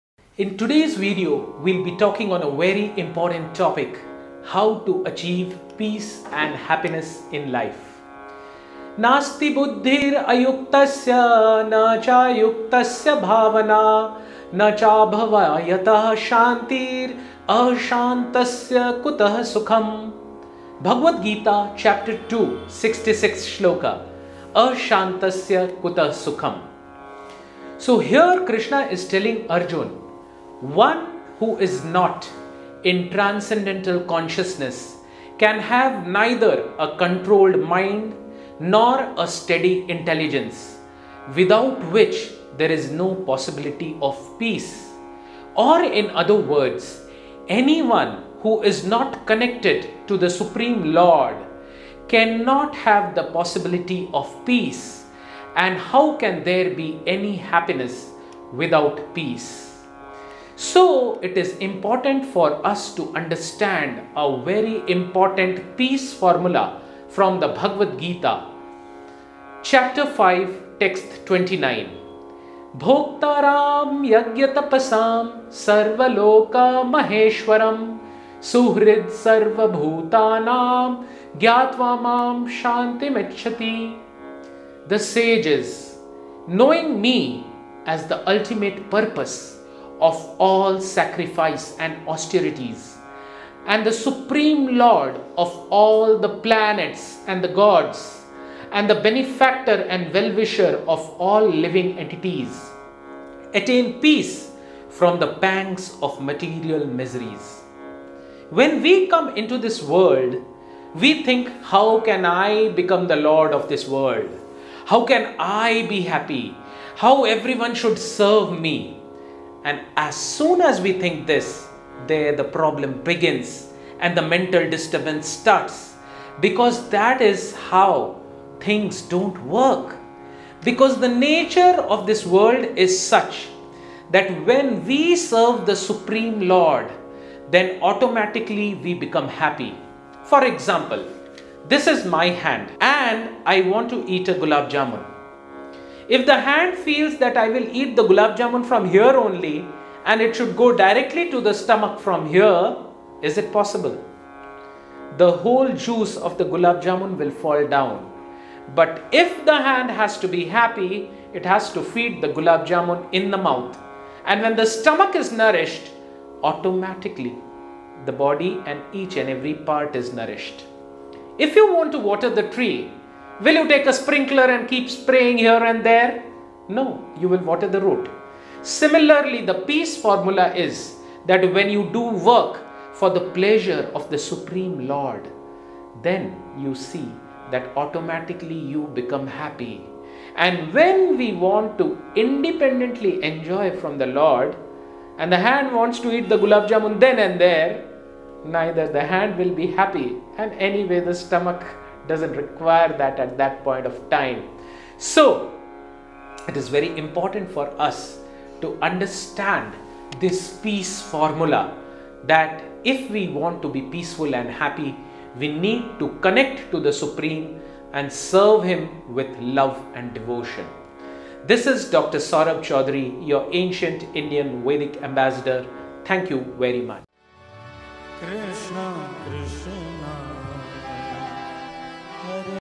How to Achieve Peace And Happiness in Life | HG Rampriya Prabhu
Rampriya_Das iskconranchi How to achieve peace and happiness in life| HG Rampriya Prabhu
🙏 हरे कृष्ण
आप सभी  को ISKCON RANCHI OFFICIAL YOUTUBE CHANNEL  की और से
 | इस वीडियो में हम जानेंगे |